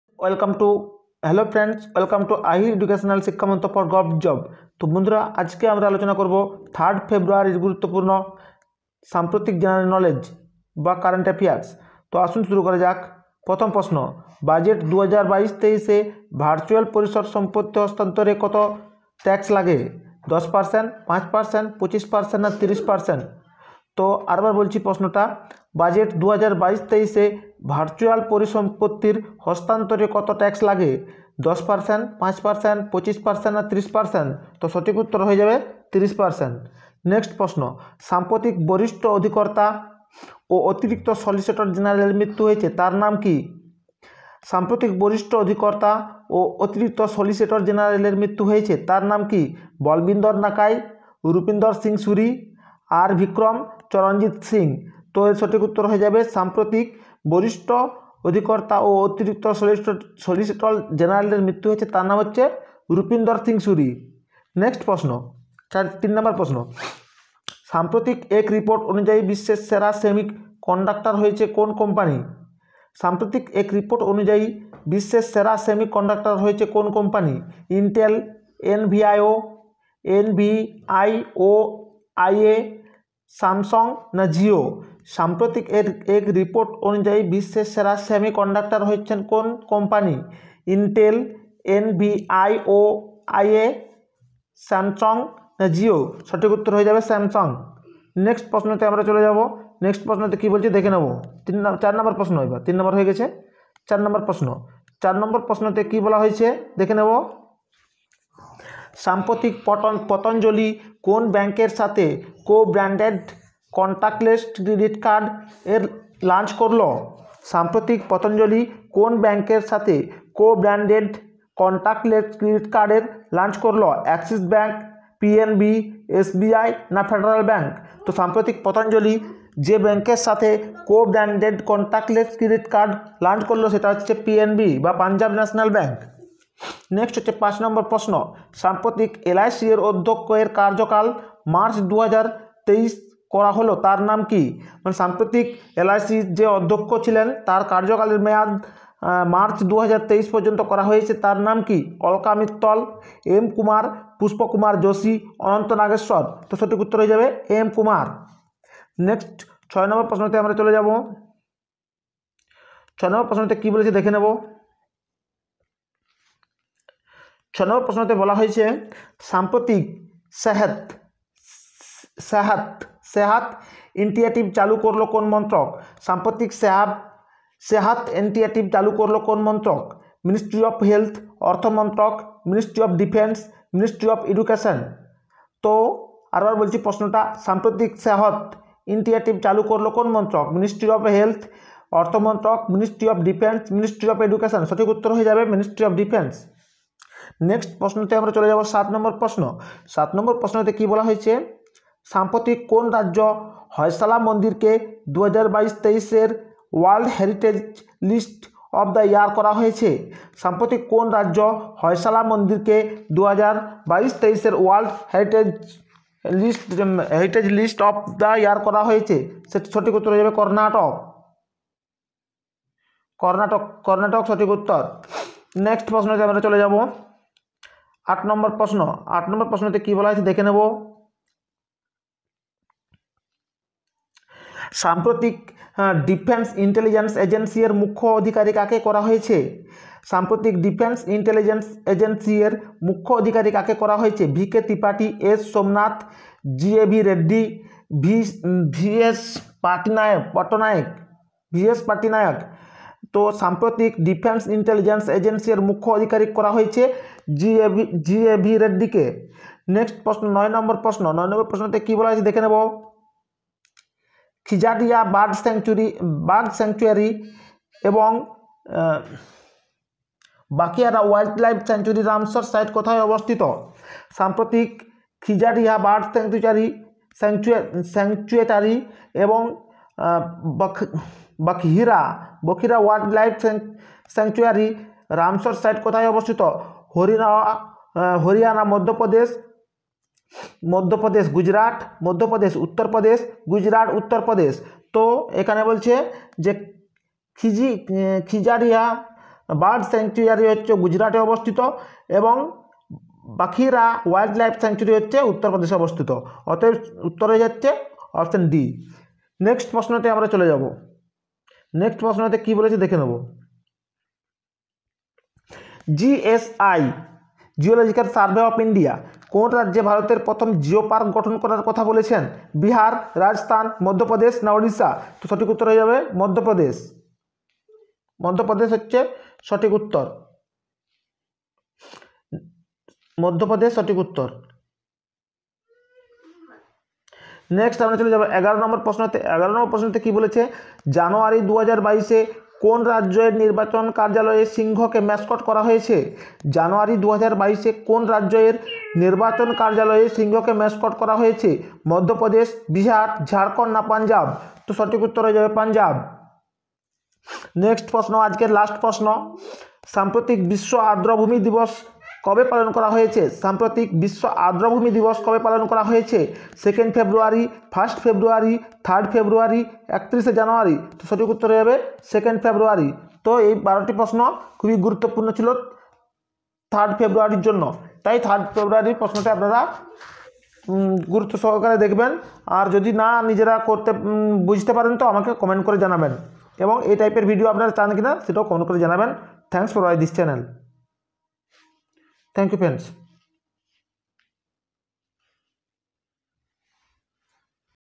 Current affairs Bengali 3rd February 2022 Latest Multiple type Question and Answer
This video I share top Current affairs 3rd February 2022  in Bengali Language if Need more regular Video please stay tune Ahir educational siksha mantra for govt job.